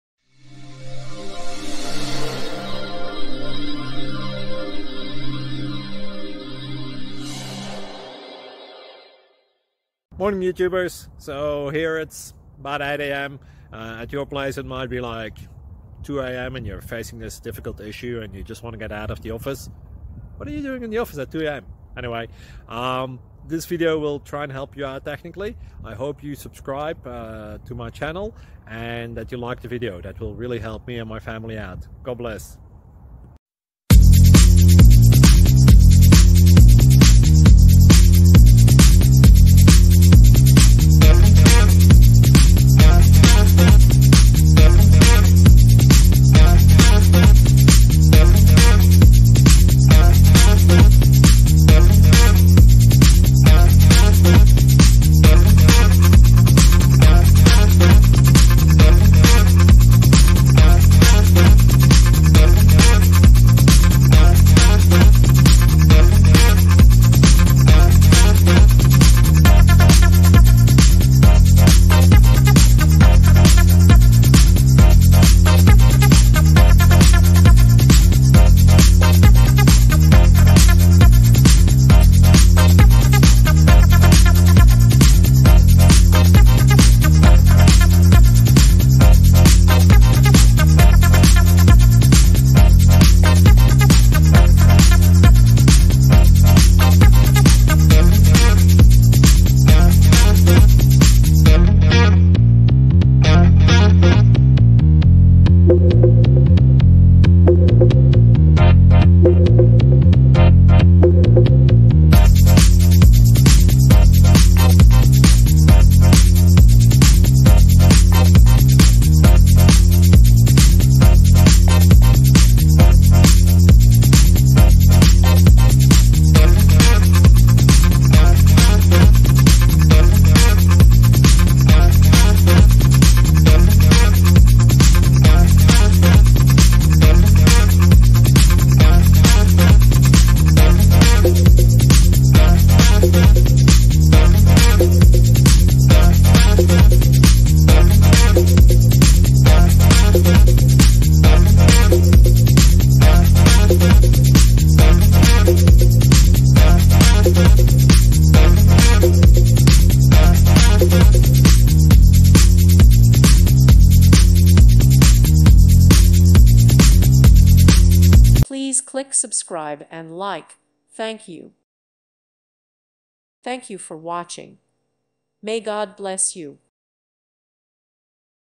Code Review: Cleaning up a basic Python script that takes and recites a pizza order (2 Solutions!!)
Code Review: Cleaning up a basic Python script that takes and recites a pizza order

With thanks & praise to God, and with thanks to the many people who have made this project possible! You are responsible for your own actions.